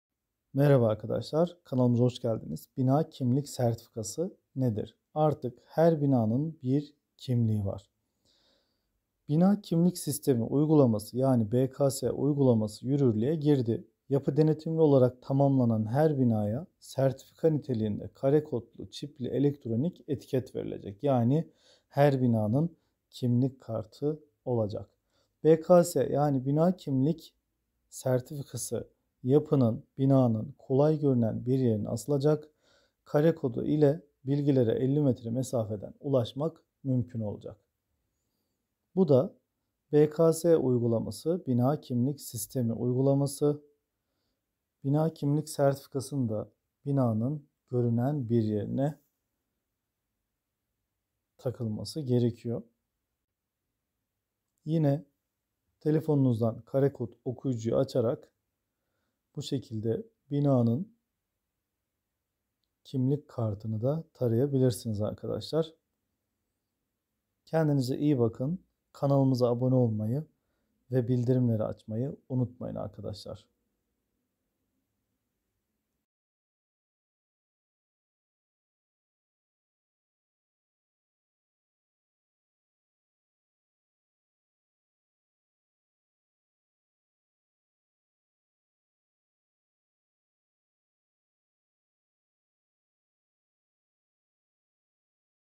bina kimlik sertifikası nedir
Bina Kimlik Sertifikası 4708 sayılı Yapı Denetimi Hakkında Kanun kapsamında tamamlanan binaların teknik ve genel bilgilerine, Bakanlıkça farklı modüllerde yapılan yetkilendirmelerle, hem yapının sahibi vatandaşlarca hem de sonraki aşamalarda kamu görevlileri tarafından ulaşılması amacıyla geliştirilen bir uygulamadır.

Herhangi bir ücret alınmayacak. Çevre ve Şehircilik Bakanlığınca Yapı Denetimi Uygulama Yönetmeliğinde Değişiklik Yapılmasına Dair Yönetmelik Resmi Gazete'de yayımlandı. Yapılara kimlik belgesi verilmesi niteliğinde olan BKS ile her yapıya sertifika niteliğinde QR kodlu ve RFID çipli elektronik etiket verilecek.